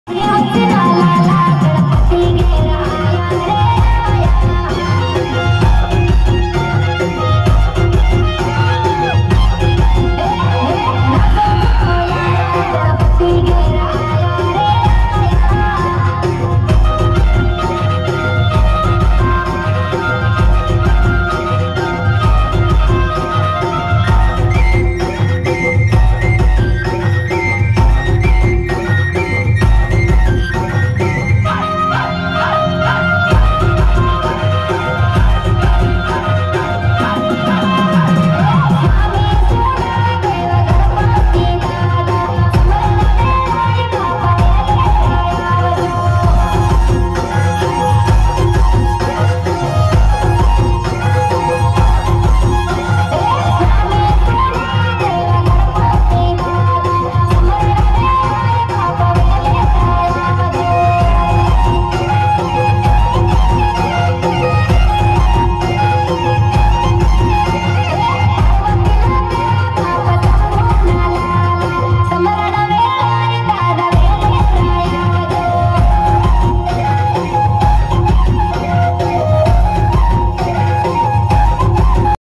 आदिवासी गणपति उत्सव डांस आमला 2021 // adivasi ganpati utshv Amala 2021 // Night Video Ganpati Amala
adivasi ganpati utshv dance amala 2021 // Night Video